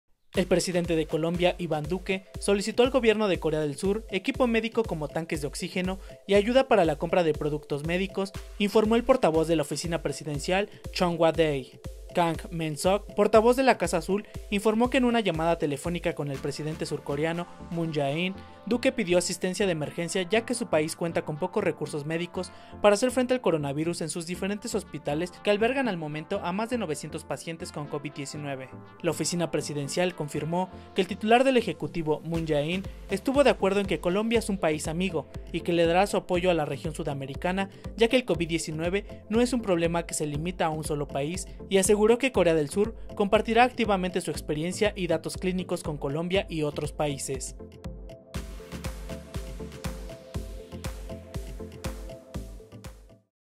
Colombia urge a Corea del Sur apoyo con material médico
El presidente de Colombia, Iván Duque, solicitó  al gobierno de Corea del Sur equipo médico como tanques de oxígeno y ayuda para la compra de productos médicos,  informó el portavoz de la oficina presidencial de Cheong Wa Dae.